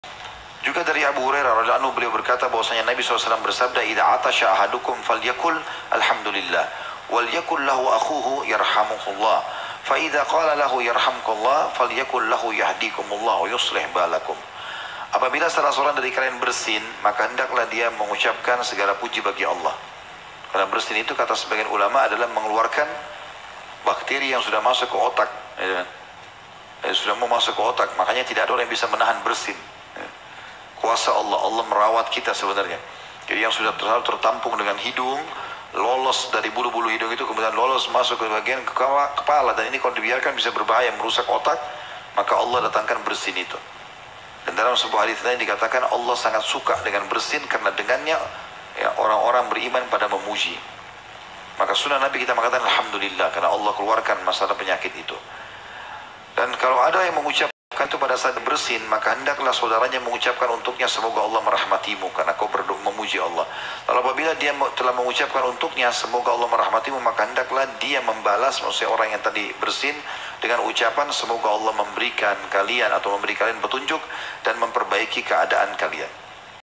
DOA KETIKA BERSIN || Ustadz Khalid Basalamah
CARA MENJAWAB BERSIN DAN ORANG YANG MENDENGARNYA...

Orang yang bersin: ALHAMDULILLAH

Bagi yang mendengarnya: YARHAMUKALLAH (bagib laki-laki),YARHAMUKILLAH (bagi perempuan)

Orang yang bersin: YAHDIKUMULLAH WA YUSLIH BAALAKUM